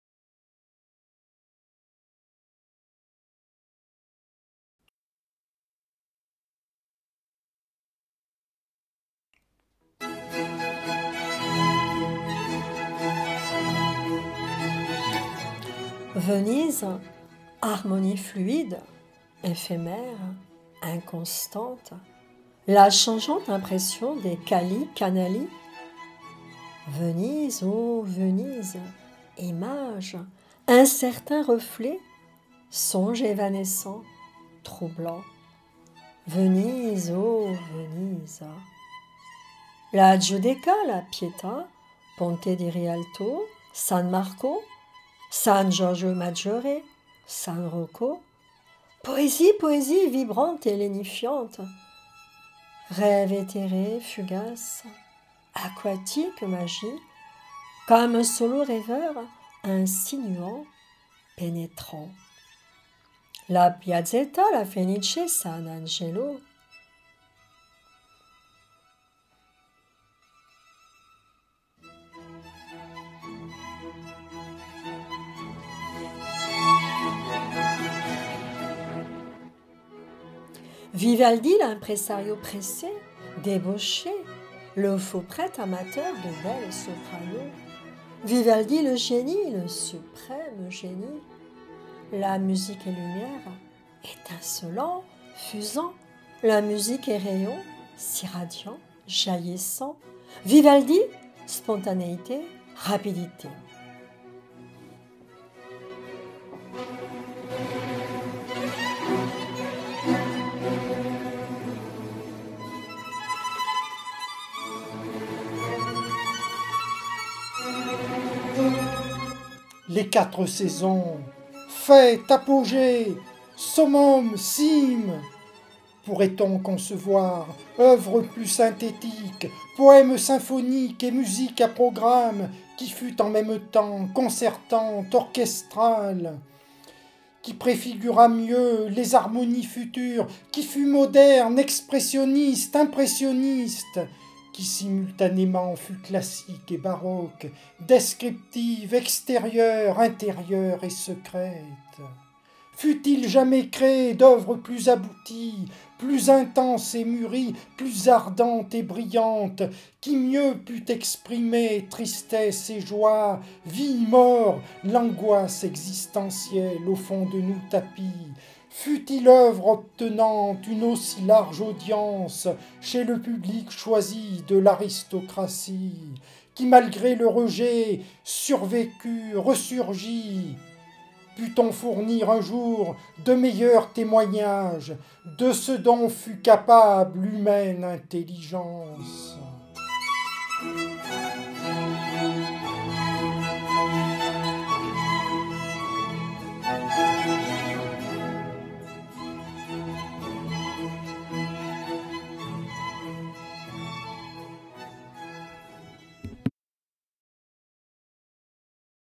Vivaldi et Venise - poème épique _ Claude Ferrandeix
Vivaldi et Venise, poème épique extrait de La Saga de l'Univers Claude Ferrandeix (Claude Fernandez)
Déclamation: Josyane Moral-Robin & Claude Fernandez